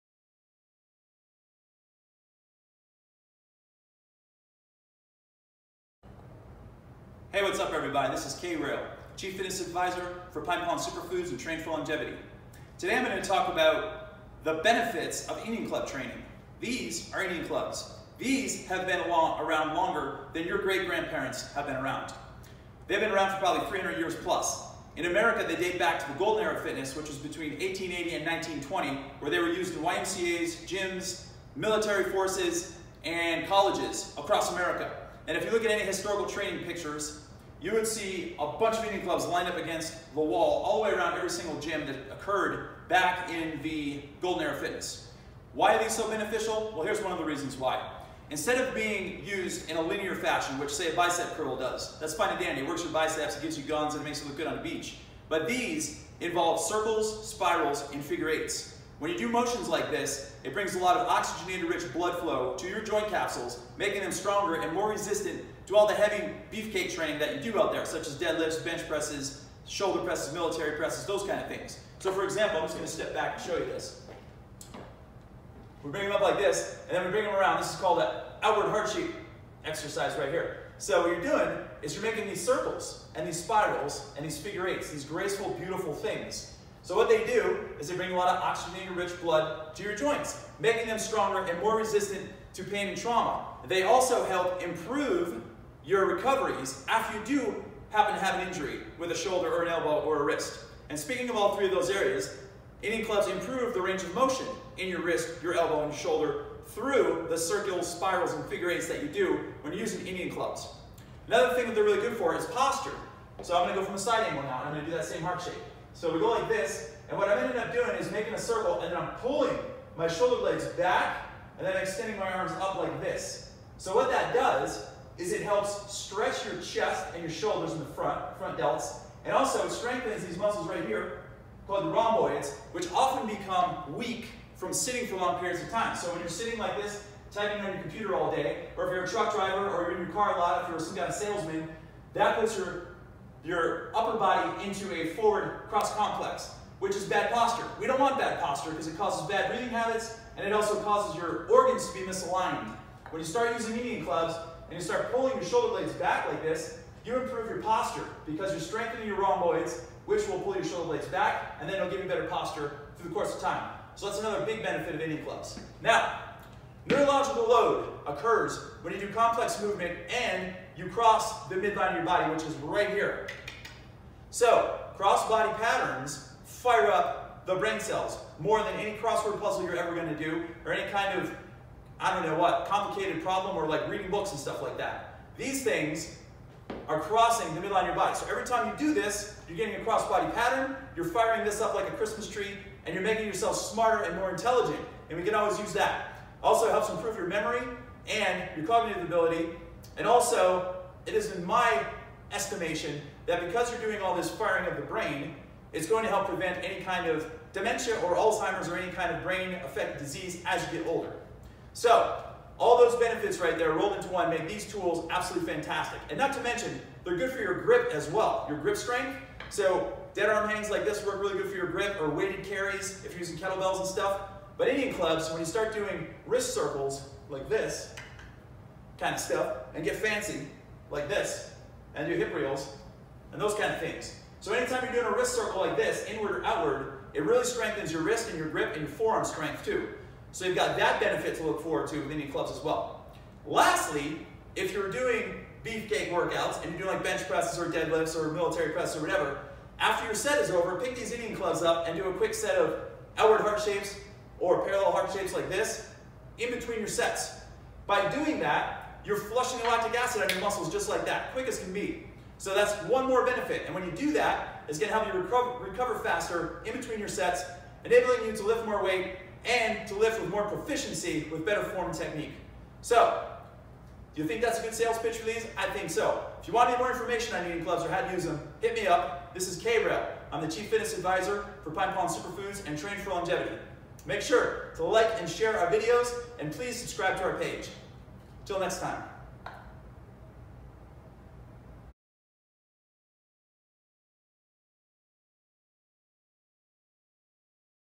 Indian Club Exercises and Tips
Indian Club Exercises and Tips, Get started and pick some up

In this video Coach Kevin discusses the benefits of Indian Clubs. He also demonstrates some Indian Club Exercises to help you get started with the basics.

Indian clubs are a great tool for improving shoulder health and mobility, while also training the brain. Use the indian club exercises discussed in the video as a warm-up and/or active recovery tool.